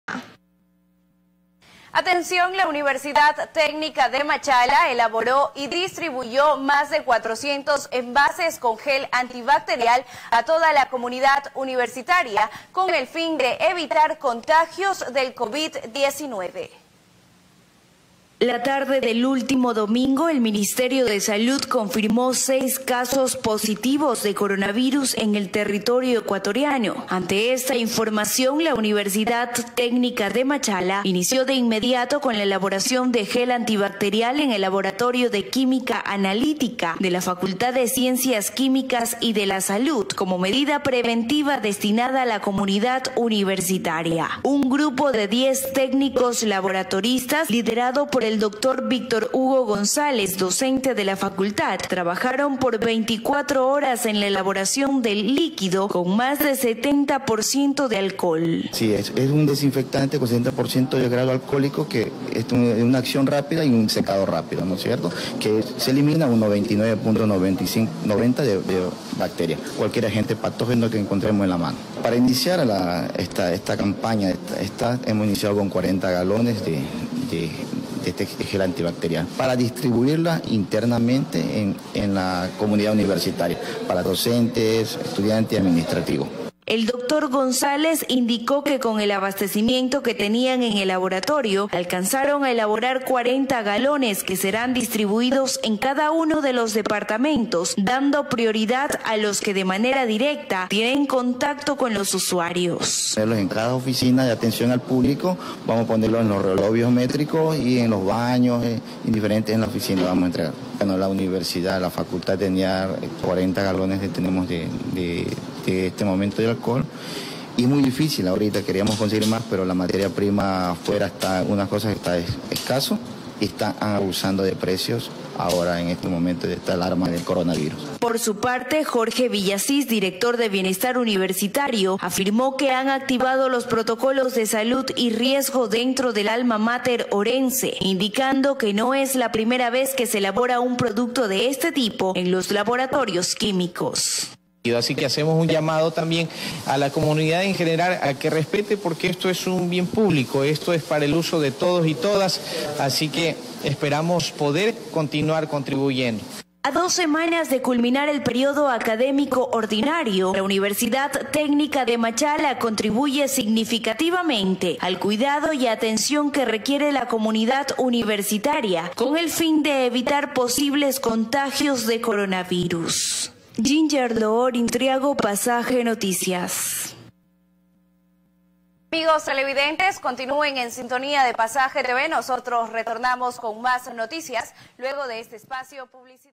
PASAJE TV DIFUNDE INFORMACIÓN SOBRE ELABORACIÓN DE GEL ANTIBACTERIAL EN UTMACH
UTMACHenMedios Agradecemos a Pasaje TV y a nuestra egresada del la carrera de Comunicación, Ginger Loor Intriago, por la cobertura informativa sobre la elaboración y distribución del gel antibacterial como medida preventiva frente al coronavirus.